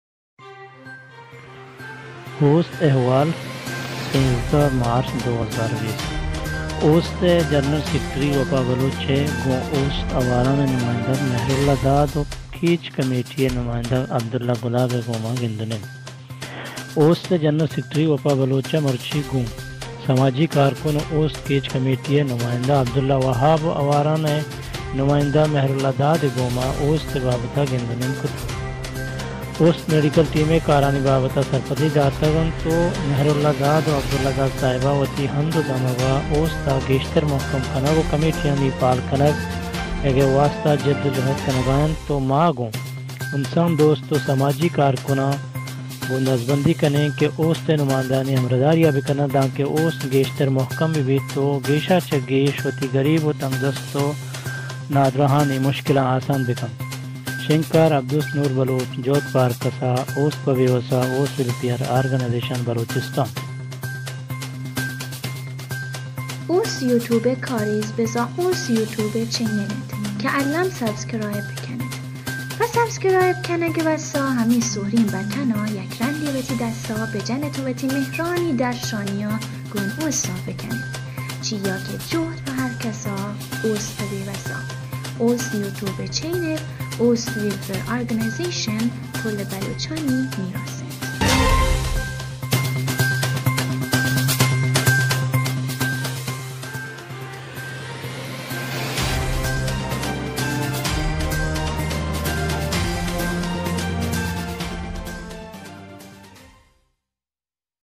اوست اھوال 13 مارچ 2020 Ost welfare organization
*اوست اھوال*Ost welfare Org  reprot
13/03/2020
*اوست ءِ جنرل سیکرٹری وپا بلوچ ءِ گوں اوست آواران ءِ نمائندہ مھر اللہ مراد ءُ کیچ کمیٹی ءِ نمائندہ عبداللہ گلاب ءِ گوما گند ءُ نند*

اوست ءِ جنرل سیکرٹری وپا بلوچ ءَ مرچی گوں سماجی کارکن ءُ اوست ءِ کیچ کمیٹی ءِ نمائندہ عبداللہ گلاب ءُ آواران ءِ نمائندہ مھراللہ مراد ءِ گوما اوست ءِ بابت ءَگند ءُ نند کتگ۔
اوست میڈیکل ٹیم ءِ کارانی بابت ءَ سرپدی داتگ انت۔
مھر اللہ مراد ءُ عبداللہ گلاب ساہب ءَ وتی ھند ءُ دمگ ءَ اوست ءِ گیشتر مھکم کنگ ءُ کمیٹیانی فعال کنگ ءِ واستہ جد جھد کنگ ءَ انت ما گوں انسان دوست ءُ سماجی کارکناں گوں دزبندی کن ایں اوست ءِ نمائندہانی ھمراہداری ءَ بکن ات دانکے اوست گیشتر مھکم بہ بیت گیش ءَ چہ گیش گریب ءُ تنگدستانی مشکل آسان بہ بنت۔

*شنگکار: عبدوست نور*
*جھد پہ ھر کسءَ*
*اوست پہ بیوسءَ*
*اوست ویلفیئر آرگنائزیشن بلوچستان*